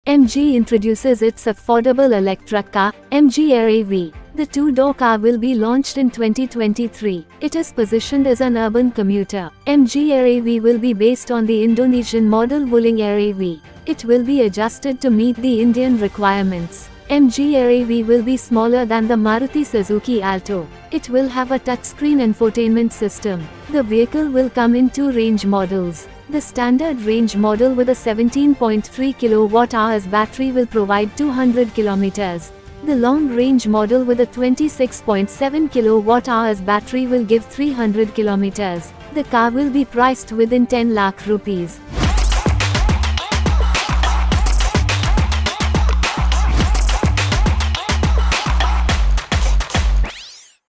MG's Affordable Air EV to Hit the Indian Market in 2023
MG introduces its affordable electric car, MG Air EV
The two-door car will be launched in 2023
It is positioned as an urban commuter
MG Air EV will be based on the Indonesian model Wuling Air EV
It will be adjusted to meet the Indian requirements
MG Air EV will be smaller than Maruti Suzuki Alto
It will have a touchscreen infotainment system
The vehicle will come in two range models
The standard range model with a 17.3 kWh battery will provide 200 km
The long-range model with a 26.7 kWh battery will give 300 km
The car will be priced within Rs 10 lakhs